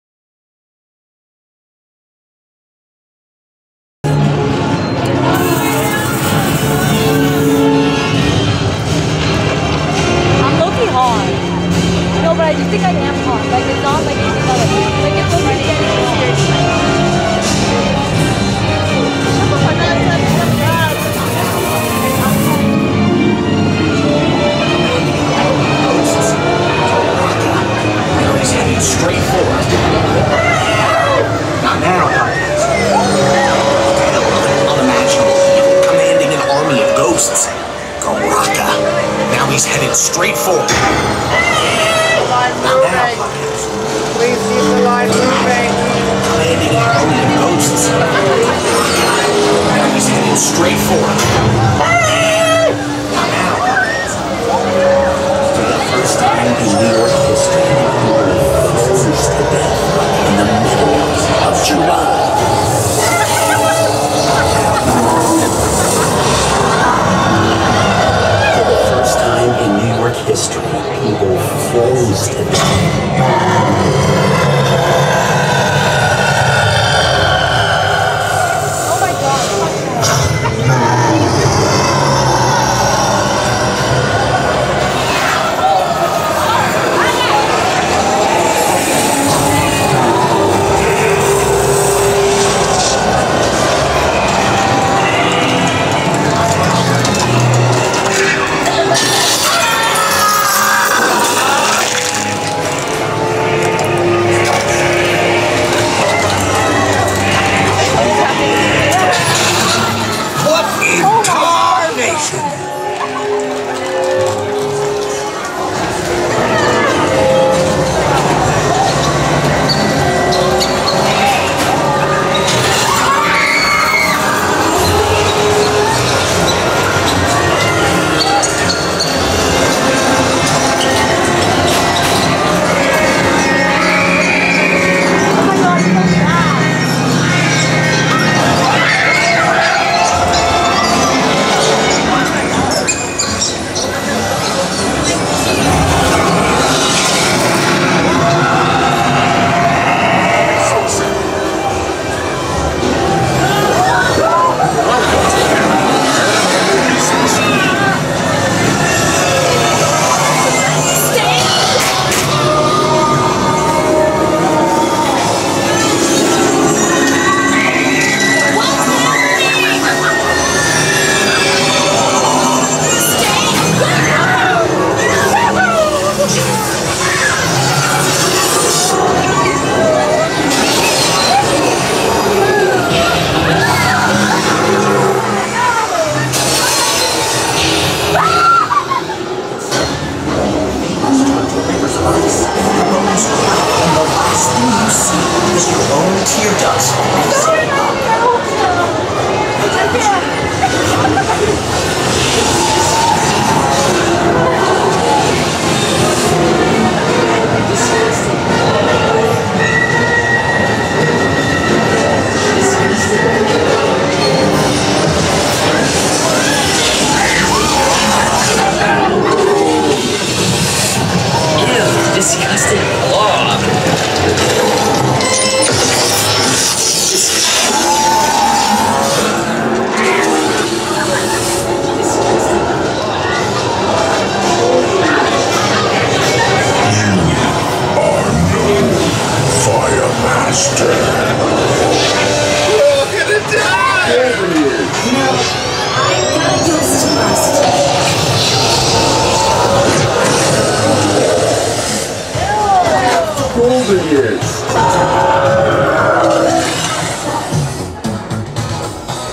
Ghostbusters: Frozen Empire Haunted House Walkthrough | Universal Studios Hollywood HHN
"A bone-chilling new specter, Garraka, has escaped from an ancient artifact. He’ll stop you cold with a whole army of ghosts. Try not to freeze in fear as you dodge spears of ice and he freezes people solid before your eyes. The Ghostbusters must team up to save the world from a second Ice Age."

Universal Studios Hollywood Halloween Horror Nights